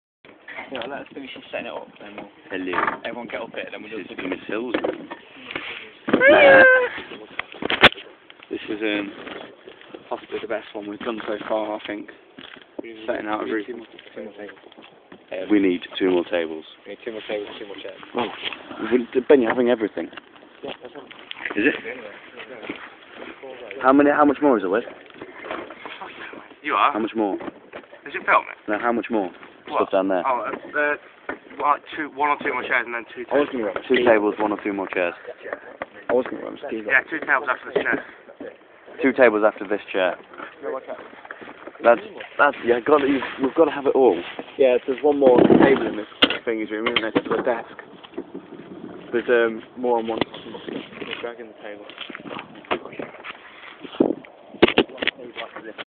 JTHS VI Form Last Day- Unseen Footage - Mrs Hill's Roof Room
The aptly named "Night of Death". well there was no actual death, but you get my point. Well it's 1.00am-ish, after breaking into school earlier on, the very origional idea of moving a classroom onto the roof took place, here's a wee snipet of how the illusion was created.